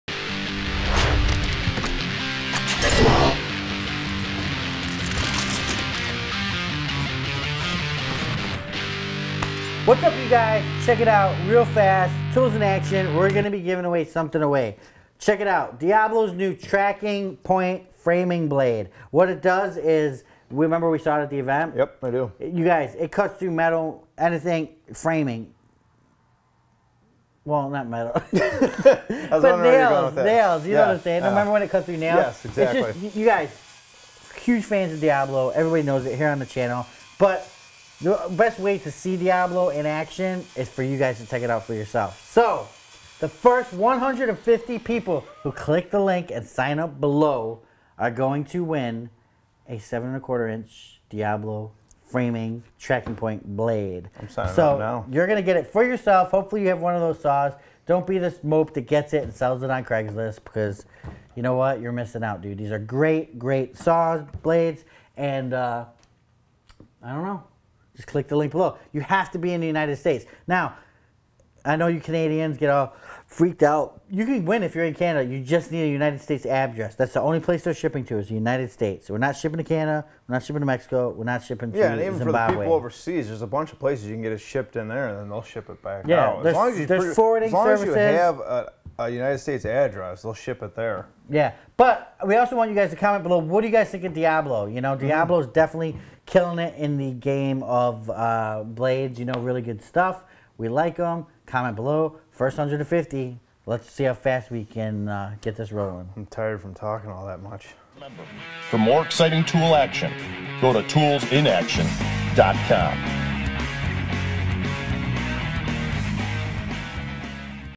Diablo Tracking Point Circular Saw Giveaway - 150 Winners
Diablo is hooking up the first 150 TIA crew members with a free 7 1/4" saw blade sample. You must have a USA address.  Follow